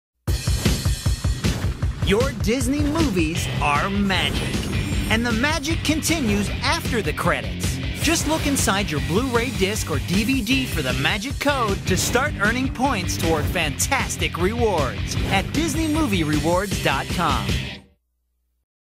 LX Movie Rewards Promo (2nd Update) (V3)
Here’s the 3rd version of my newest update for the LX Movie Rewards promo, which would be used on many LX Blu-Rays and DVDs in the JMTD AU from 2017 to 2018.